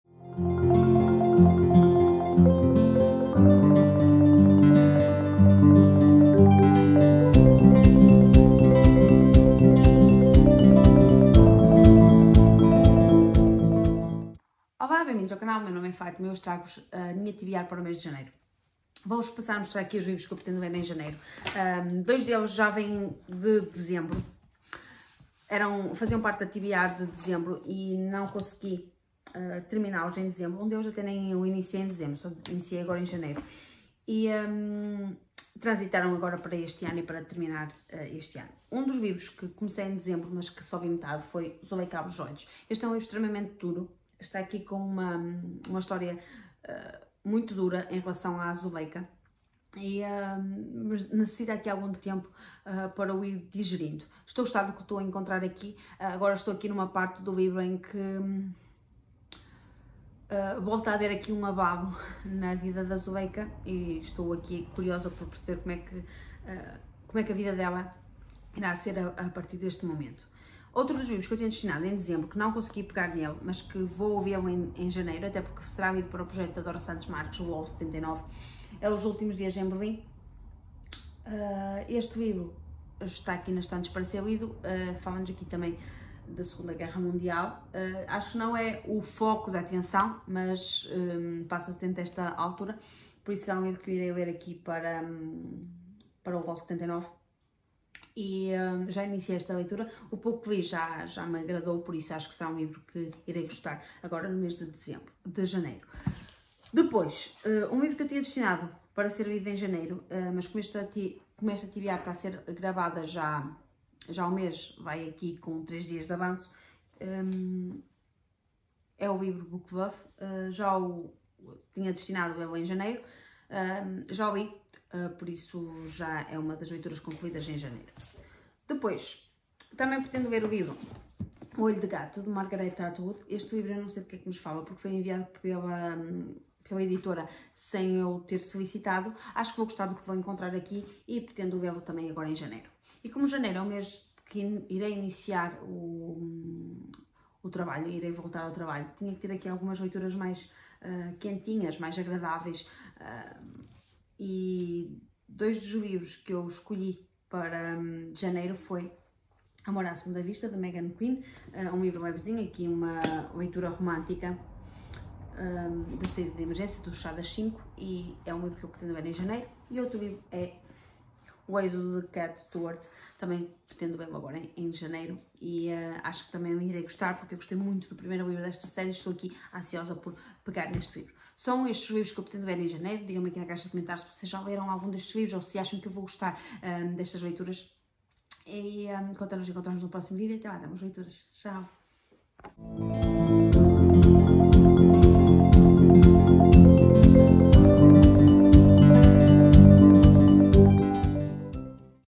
TBR Janeiro
Leituras que pretendo realizar em Janeiro.